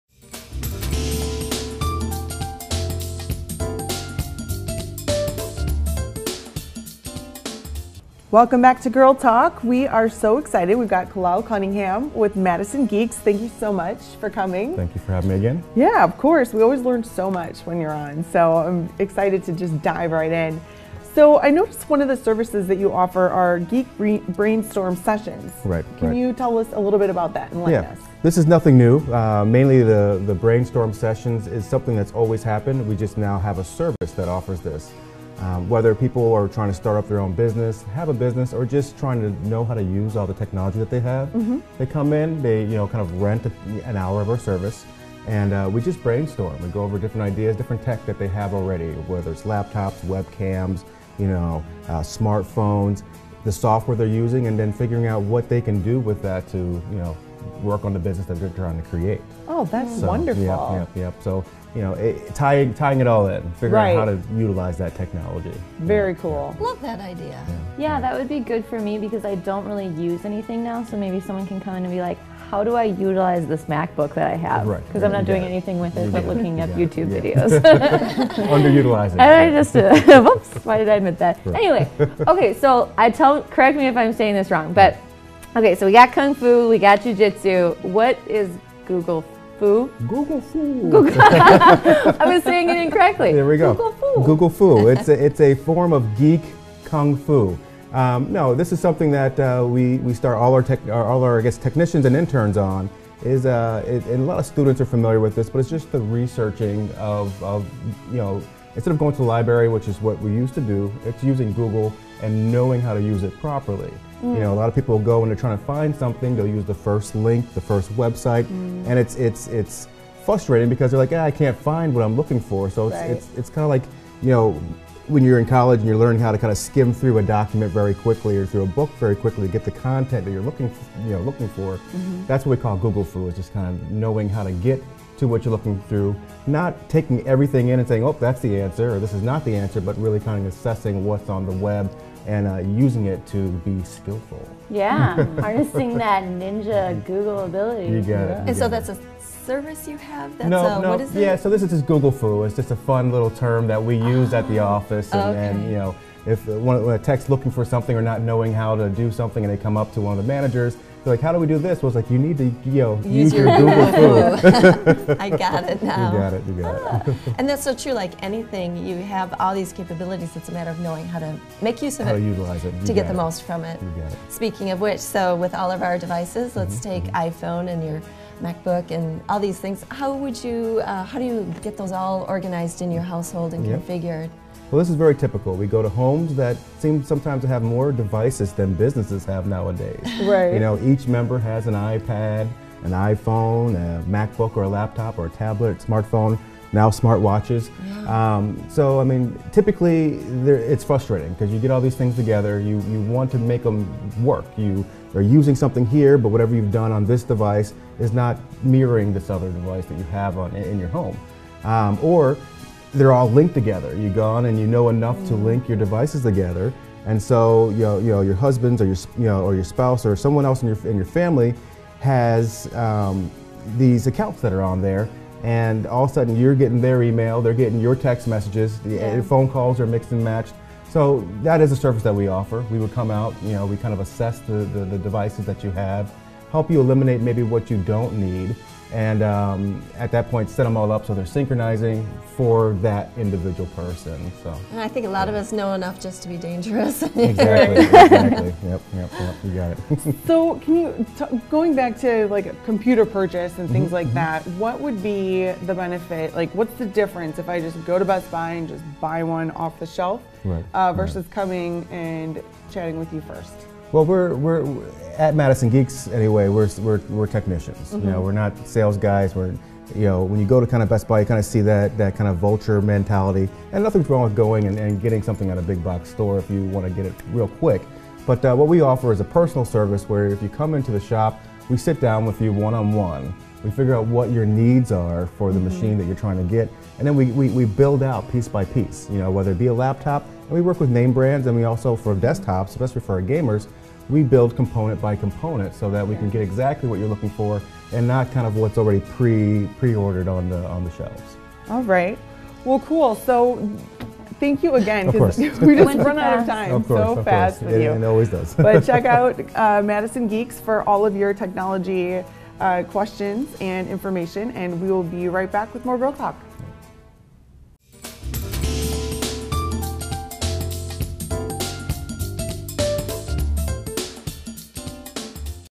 Girl Talk | MadisonGeeks.com | Madison Geeks | 4/24/15
Madison Geeks Inc.
Kahlal Cunninghan
President

What is the topic: Geek'n Out, with Madison Geeks!

Question #1: We noticed one of the services Madison Geeks' offers is, Geek Brainstorm Sessions. Please enlighten us.
Question #2: I've heard of Kung-Fu and even Ju Jit Su... But what is GoogleFoo?? o_O
Question #3: iPhone, iPad, Macbook, oh my... what's the best way for a household to setup & configure ALL their technology!??
Question #4: What's the difference from buying a computer from say, Best Buy, or ordering one from over at Madison Geeks?
Question #5: Android or iPhone, which device should I get next?
Question #6: Should we be excited about the new... Apple Smartwatch??